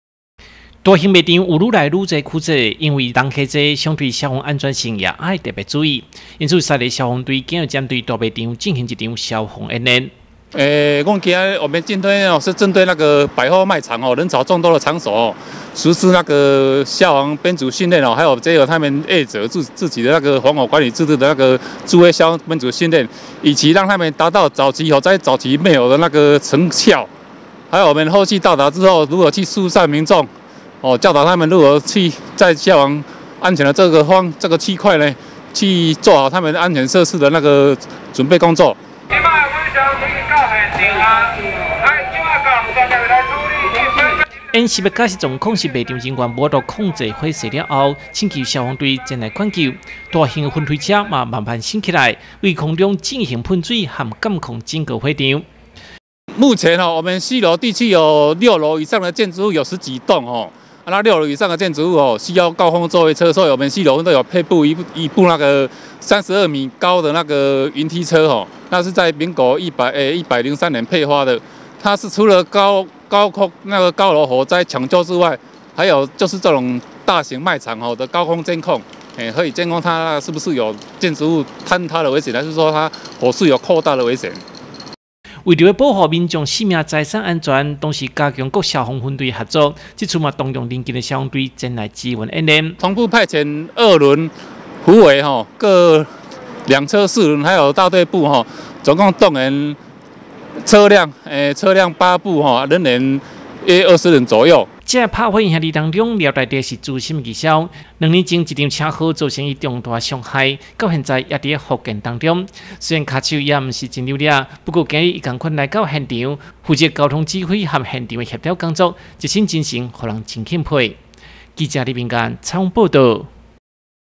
雲林新聞網-大賣場興起 西螺進行消防災害演練
針對賣場越蓋越大、人潮聚集，消防安全問題，相對也越來越重要，西螺消防分隊3/11就在西螺一間新啟用不久的綜合商場，進行一場大型消防演練。